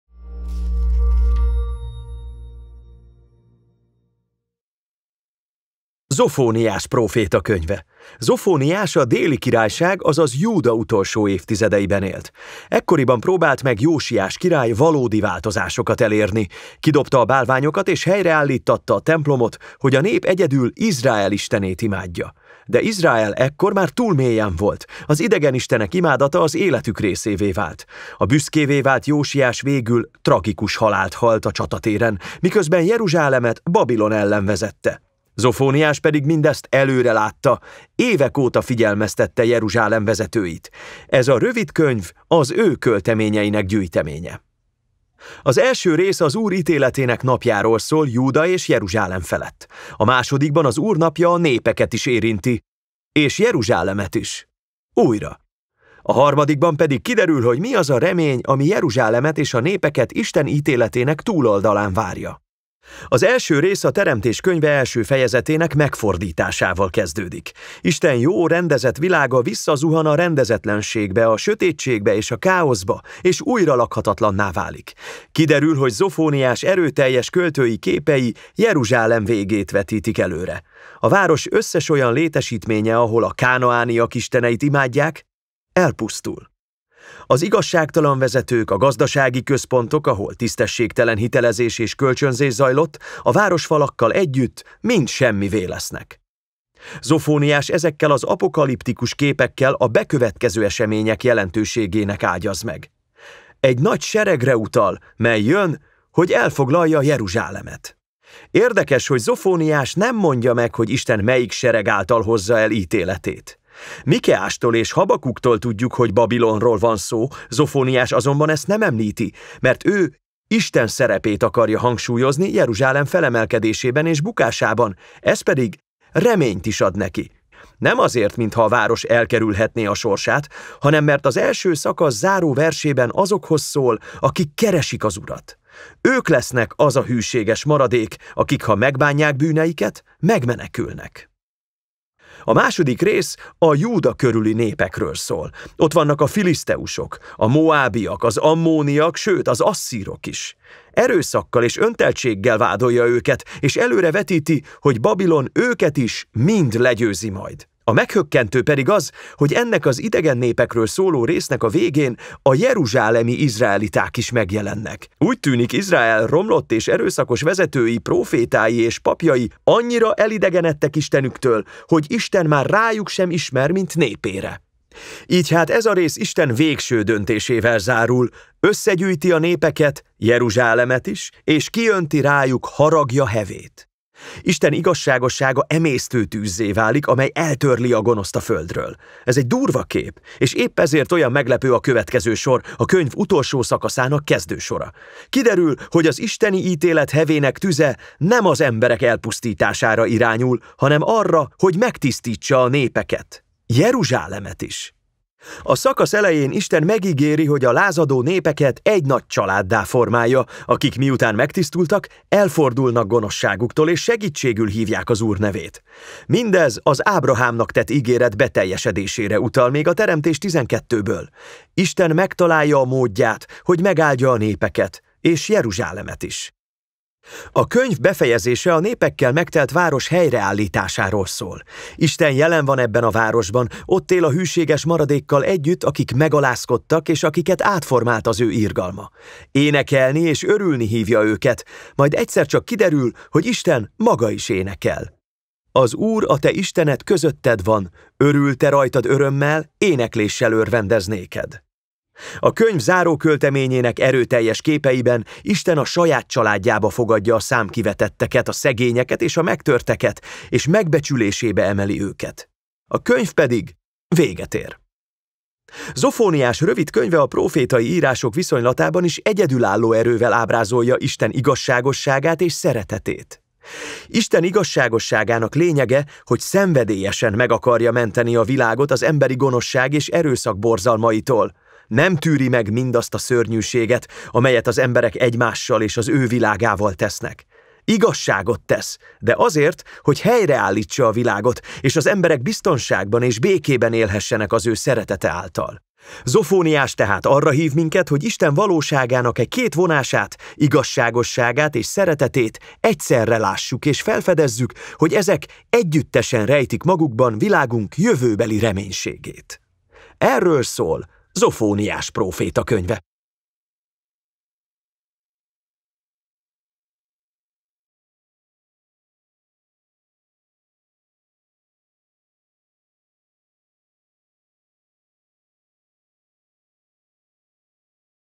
Áttekintés: Zofóniás
Zofóniás Isten mindent megtisztító ítéletét hirdeti Izráelnek, amely véget vet a gonosznak, és egy új jövőt tár fel, melyben minden ember békében élhet.

A BibleProject eredeti, angol nyelvű videóját a Fordított Királyság Alapítvány fordította magyarra.

Fordító: Abrudan Katalin
Lektorok: Fodor István, Horváth-Kávai Andrea, Szalkay-Lőrinc Ágnes, Székely Pál Domokos, Veres Anna
Színművész: Zámbori Soma
Hangmérnö: Pap Gergely
Grafikus: Balog István
Animátor: Bötkös Tamás